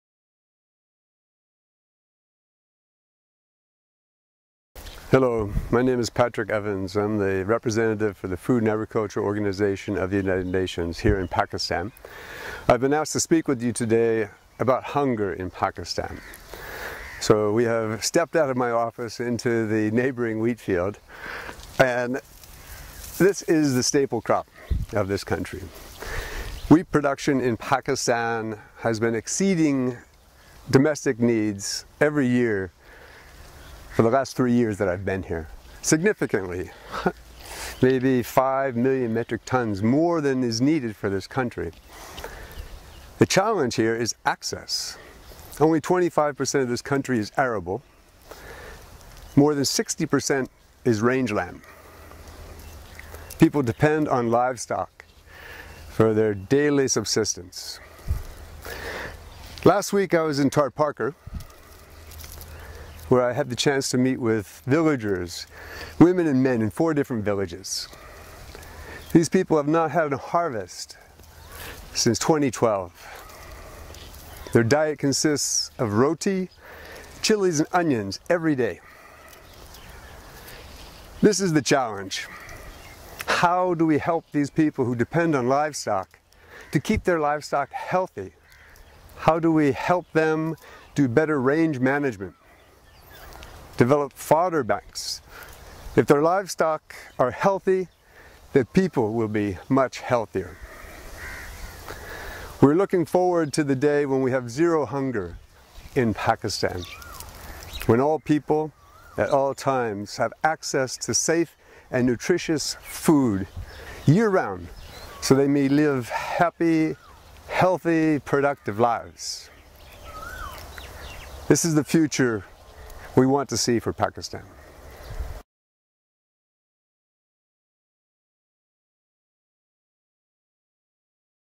How to achieve Zero Hunger in Pakistan?
Wheat production in Pakistan has surpassed domestic needs significantly at least over the last 3 years but even now, there are areas that have not had a harvest.
What are the challenges to ensure Zero Hunger in Pakistan?
What kind of future does FAO want to see for Pakistan?
Here’s a message from FAO Representative in Pakistan, Patrick T. Evans